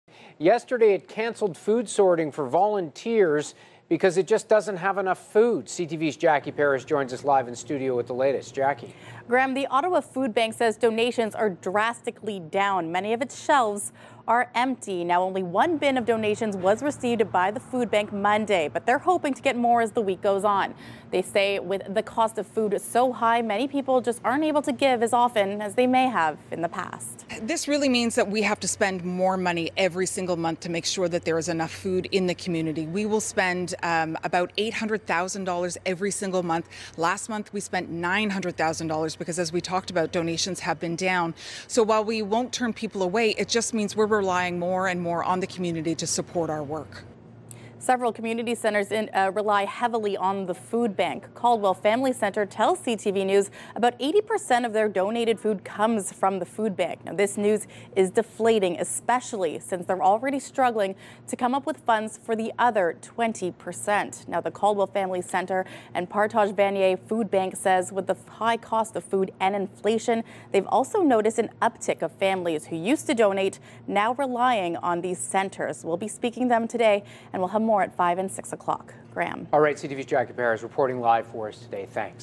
Ottawa food bank cancels volunteer shifts due to lack of donations
The Ottawa Food Bank has cancelled volunteer sorting shifts due to a lack of food donations, CTV's Jackie Perez reports.